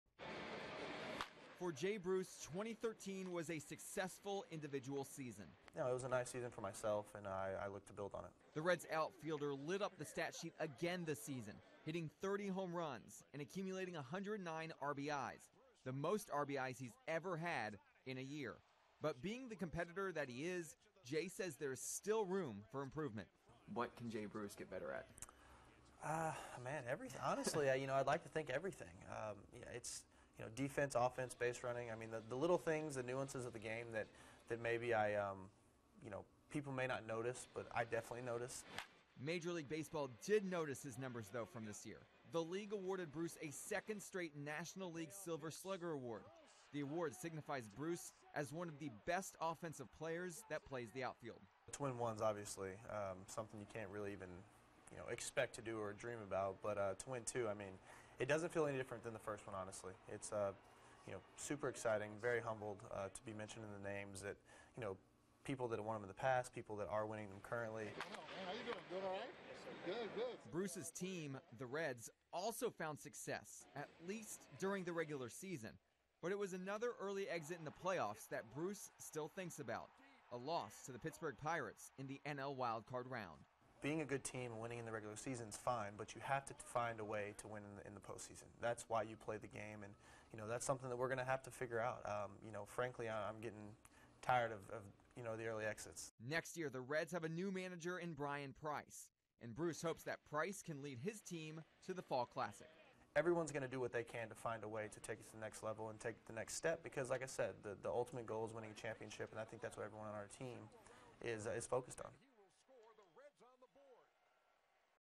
Baseball player from Beaumont discusses latest honor
This time of year involves giving gifts and ultimately receiving gifts, whether you think you deserve something or not.

Major League Baseball gave Beaumont native and current Cincinnati Reds outfielder Jay Bruce a gift recently. Bruce stopped by the KFDM News studio to talk about his latest honor.